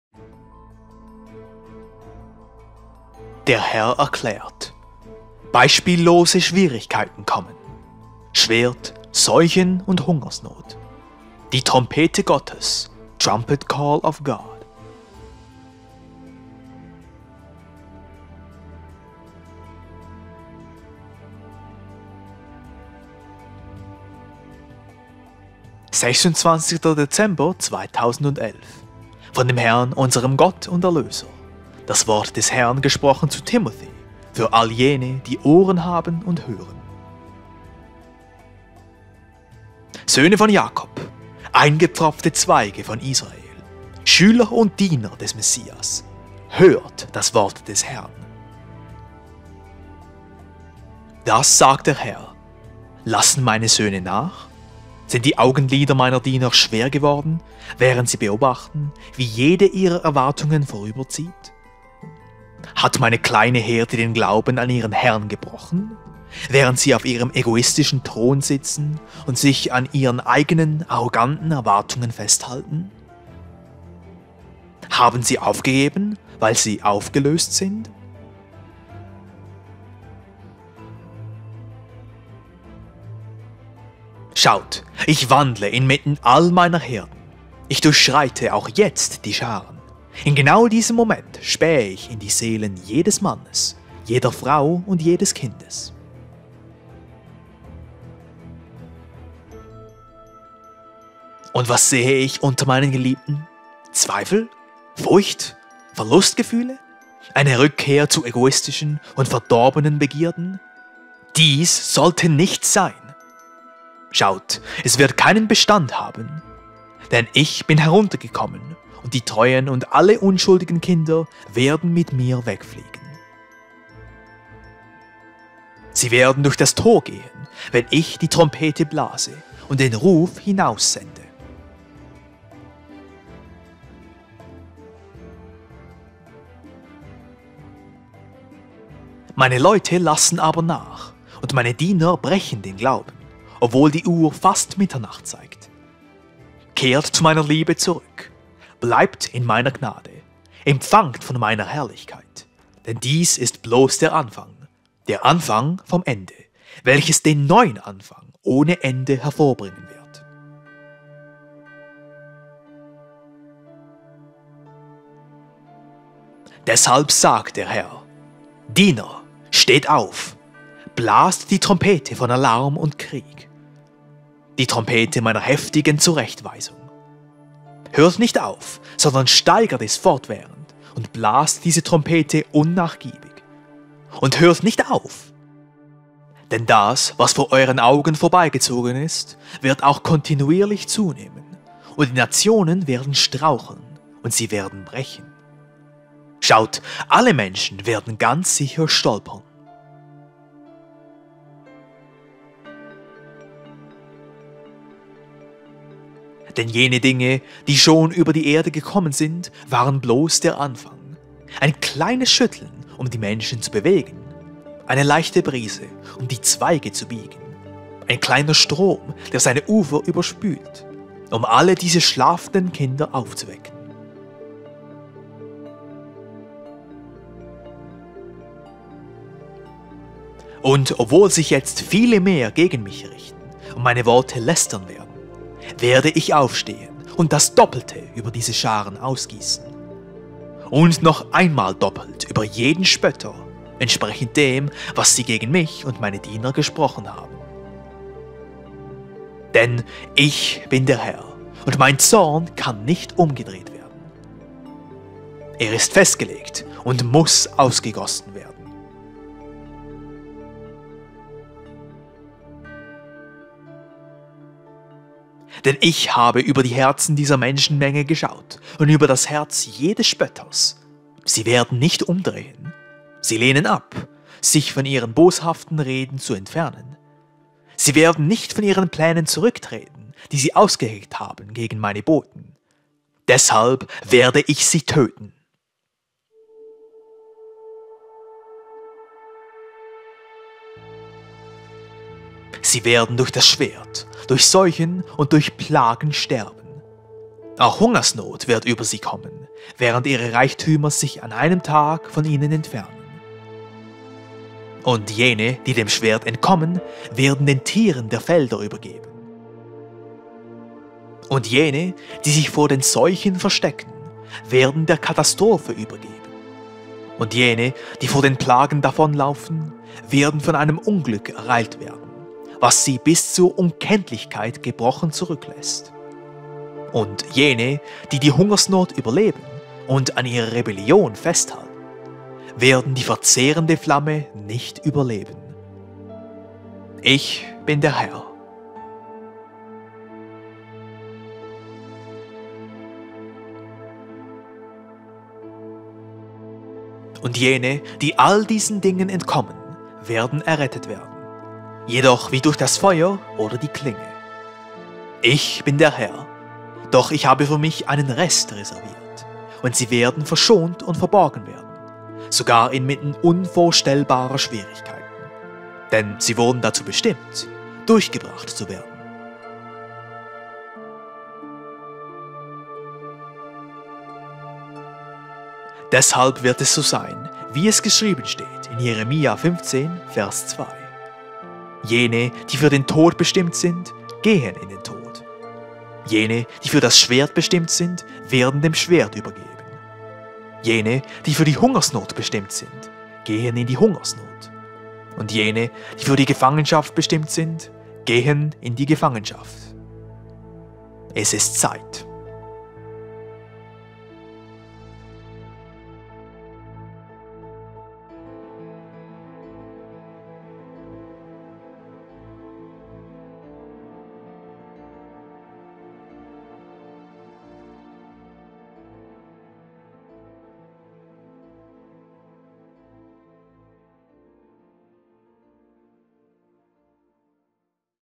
Rhema 04.12.2024 🎺 Schwert, Plagen und Hungersnot... Beispiellose Schwierigkeiten kommen
Sie werden durch das Schwert, durch Seuchen, Plagen & Hungersnot sterben

26. Dezember 2011 – Von Dem Herrn, Unserem Gott und Erlöser – Das Wort Des Herrn gesprochen zu Timothy für all Jene, die Ohren haben und hören

Söhne von Jakob! Eingepfropfte Zweige von Israel! Schüler und Diener des Messias! Hört das Wort Des Herrn!…

Das sagt Der Herr… Lassen Meine Söhne nach? Sind die Augenlider Meiner Diener schwer geworden, während sie beobachten, wie jede ihrer Erwartungen vorüberzieht? Hat Meine kleine Herde den Glauben an ihren Herrn gebrochen, während sie auf ihrem egoistischen Thron sitzen und sich an ihren eigenen arroganten Erwartungen festhalten? Haben sie aufgegeben, weil sie aufgelöst sind?…

Schaut, Ich wandle inmitten all Meiner Herden. Ich durchschreite auch jetzt die Scharen… In genau diesem Moment spähe Ich in die Seelen jedes Mannes, jeder Frau und jedes Kindes.

Und was sehe Ich unter Meinen Geliebten?… Zweifel? Furcht? Verlustgefühle? Eine Rückkehr zu egoistischen und verdorbenen Begierden?… DIES SOLLTE NICHT SEIN! Schaut, es wird keinen Bestand haben! Denn Ich bin herunter gekommen und die Treuen und alle unschuldigen Kinder werden mit Mir wegfliegen! Sie werden durch das Tor gehen, wenn Ich die Trompete blase und den Ruf hinaus sende!

Meine Leute lassen aber nach und Meine Diener brechen den Glauben, obwohl die Uhr fast Mitternacht zeigt… Kehrt zu Meiner Liebe zurück, bleibt in Meiner Gnade, empfangt von Meiner Herrlichkeit! Denn dies ist bloss der Anfang!… Der Anfang vom Ende, welches den neuen Anfang ohne Ende hervorbringen wird.

Deshalb sagt Der Herr… DIENER, STEHT AUF! BLAST DIE TROMPETE VON ALARM UND KRIEG!… Die Trompete Meiner heftigen Zurechtweisung! Hört nicht auf, sondern steigert es fortwährend und blast diese Trompete unnachgiebig! Und hört nicht auf! Denn das, was vor euren Augen vorbeigezogen ist, wird auch kontinuierlich zunehmen und die Nationen werden straucheln und sie werden brechen!… Schaut, alle Menschen werden ganz sicher stolpern!

Denn jene Dinge, die schon über die Erde gekommen sind, waren bloss der Anfang… Ein kleines Schütteln, um die Menschen zu bewegen, eine leichte Brise, um die Zweige zu biegen; ein kleiner Strom, der seine Ufer überspült, um all diese schlafenden Kinder aufzuwecken.

Und obwohl sich jetzt Viele mehr gegen Mich richten und Meine Worte lästern werden, werde Ich aufstehen und das Doppelte über diese Scharen ausgiessen!… Und noch einmal doppelt über jeden Spötter, entsprechend dem, was sie gegen Mich und Meine Diener gesprochen haben! Denn ICH BIN DER HERR und Mein Zorn kann nicht umgedreht werden!… Er ist festgelegt und muss ausgegossen werden!

Denn Ich habe über die Herzen dieser Menschenmenge geschaut und über das Herz jedes Spötters… Sie werden nicht umdrehen… Sie lehnen ab, sich von ihren boshaften Reden zu entfernen. Sie werden nicht von ihren Plänen zurücktreten, die sie ausgeheckt haben gegen Meine Boten… Deshalb WERDE ICH SIE TÖTEN!

Sie werden durch das Schwert, durch Seuchen und durch Plagen sterben. Auch Hungersnot wird über sie kommen, während ihre Reichtümer sich an einem Tag von ihnen entfernen… Und Jene, die dem Schwert entkommen, werden den Tieren der Felder übergeben und Jene, die sich vor den Seuchen verstecken, werden der Katastrophe übergeben und Jene, die vor den Plagen davonlaufen, werden von einem Unglück ereilt werden, was sie bis zur Unkenntlichkeit gebrochen zurücklässt und Jene, die die Hungersnot überleben und an ihrer Rebellion festhalten, werden die verzehrende Flamme nicht überleben. ICH BIN DER HERR.

Und Jene, die all diesen Dingen entkommen, werden errettet werden, jedoch wie durch das Feuer oder die Klinge. ICH BIN DER HERR. Doch Ich habe für Mich einen Rest reserviert und sie werden verschont und verborgen werden, sogar inmitten unvorstellbarer Schwierigkeiten. Denn sie wurden dazu bestimmt, durchgebracht zu werden.

Deshalb wird es so sein, wie es geschrieben steht in Jeremia 15:2… ‚Jene die für den Tod bestimmt sind, gehen in den Tod. Jene die für das Schwert bestimmt sind, werden dem Schwert übergeben, Jene, die für die Hungersnot bestimmt sind, gehen in die Hungersnot und Jene, die für die Gefangenschaft bestimmt sind, gehen in die Gefangenschaft’… Es ist Zeit.